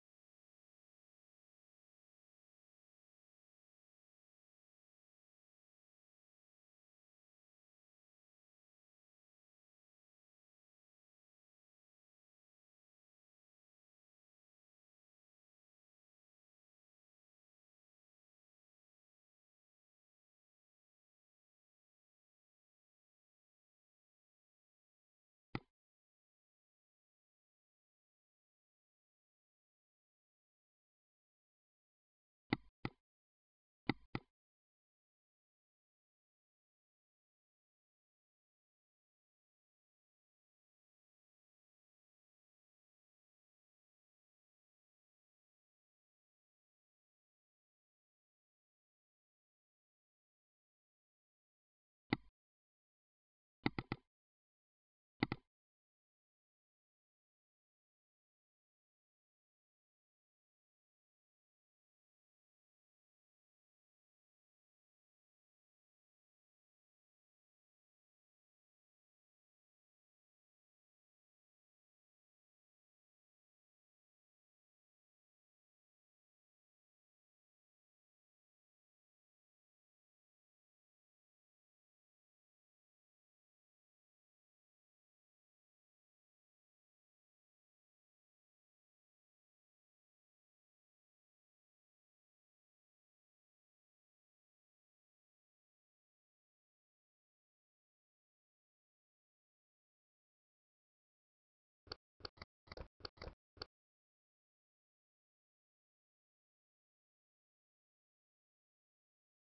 JavaScript Animated. How To Download And Install Sublime Text2 Editor
The following tutorial will show how to download and install Sublime Text editor.